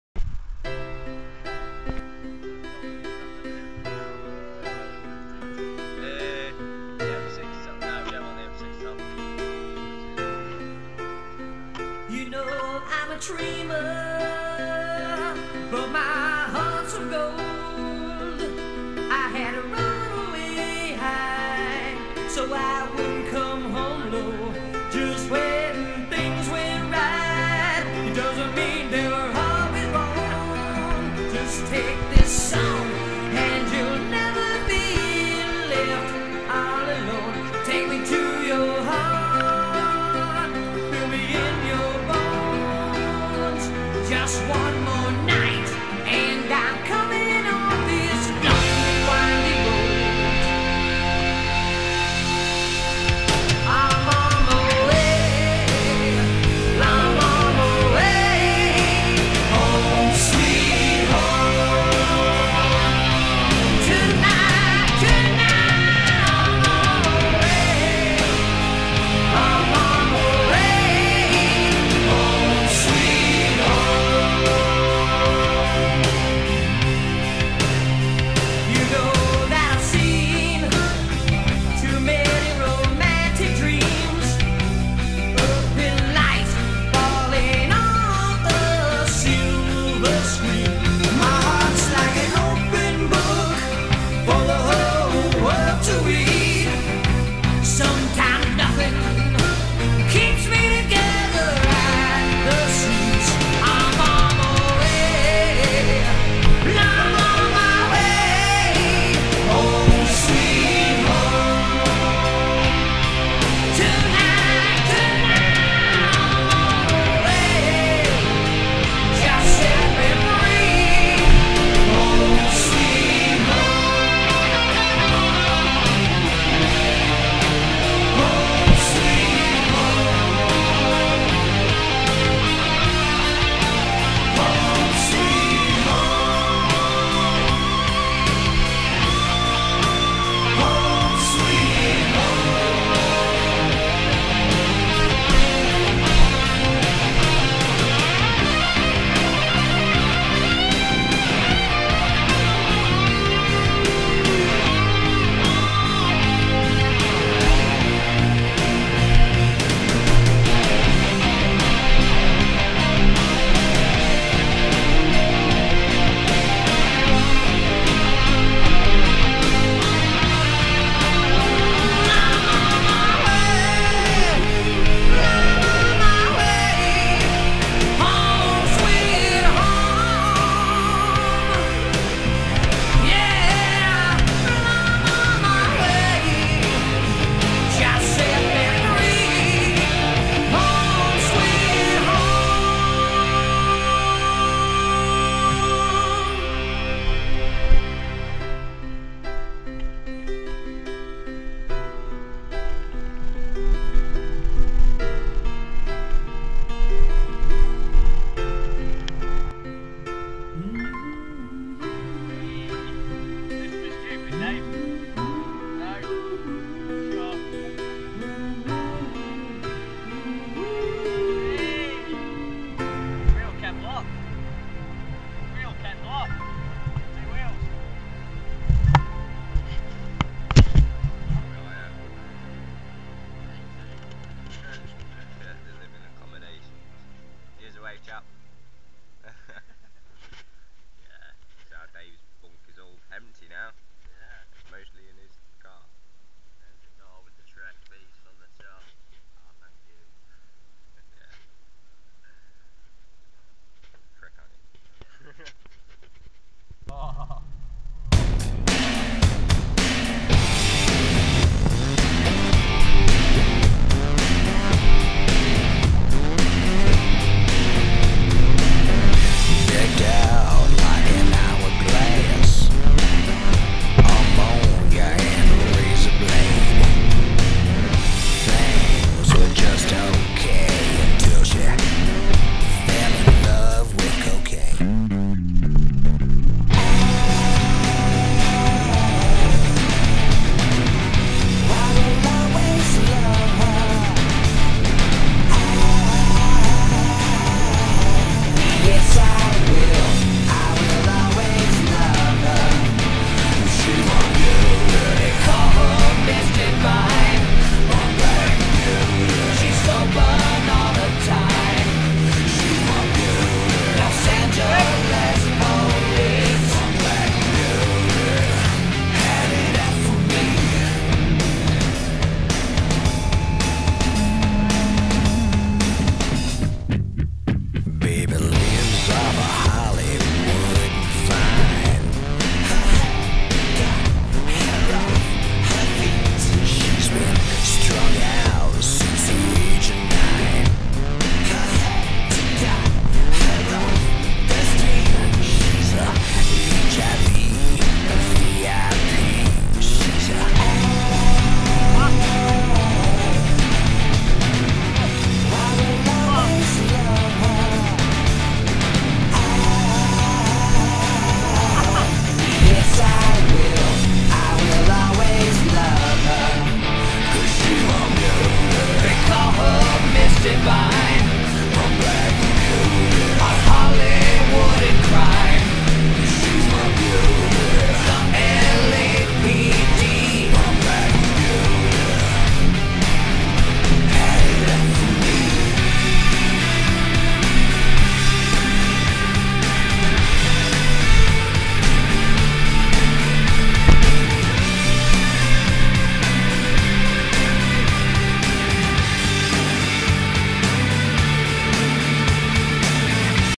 A Broken Pug Later.wmv
Short [HOONCAM] vid of the adventures of 3 lads on bikes. Its my first one... and yeah its crap but theres footage of Dan Falling off, A randomer that shared daves bunk and some cool music... its not all bad.